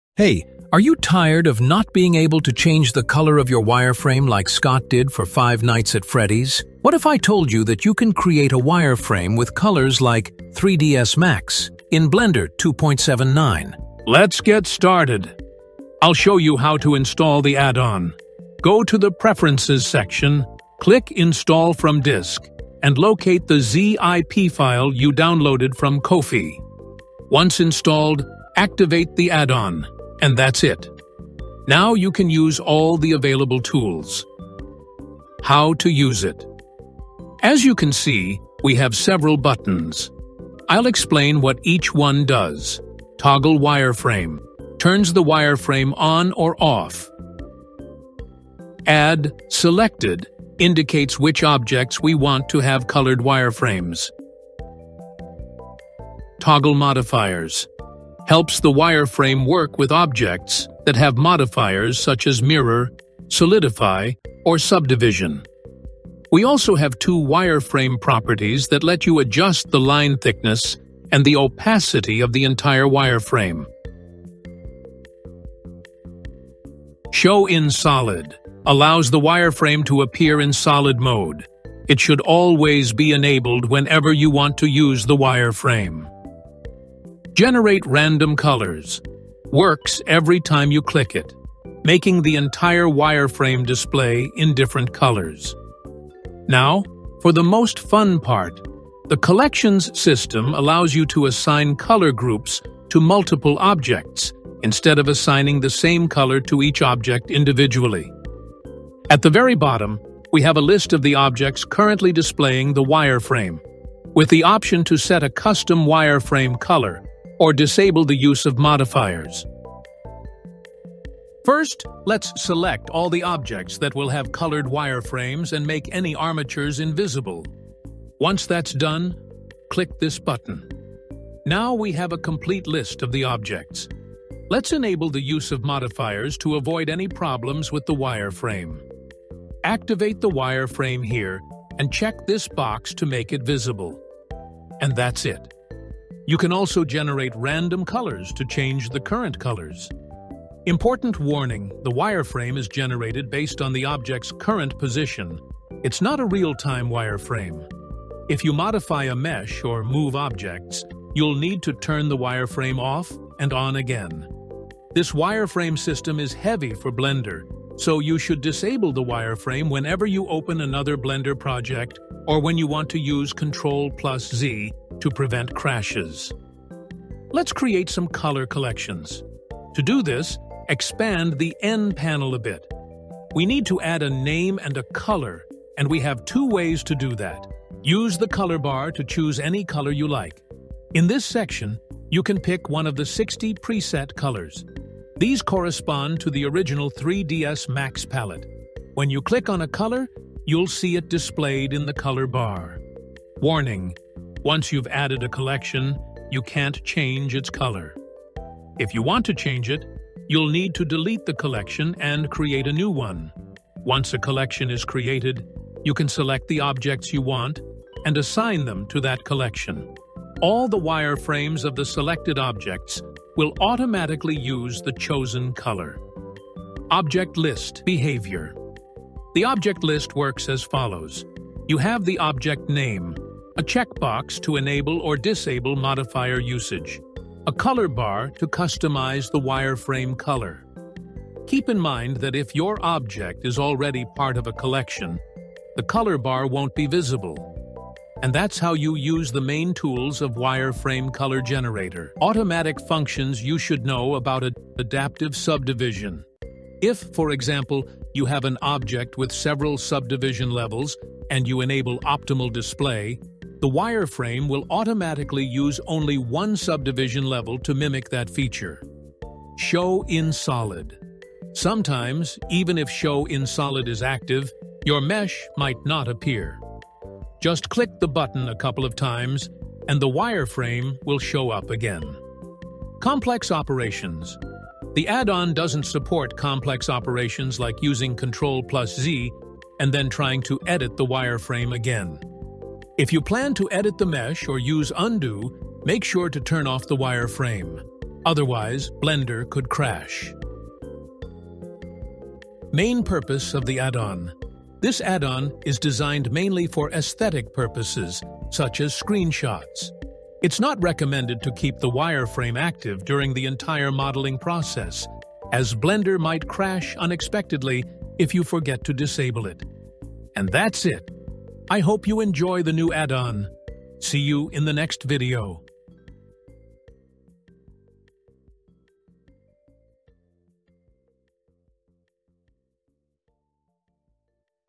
Wireframe Color Generator Tutorial
This add-on will allow you to generate a colorful wireframe exactly like the one in 3ds Max 2013. This will help your models look identical to Scott Cawthon's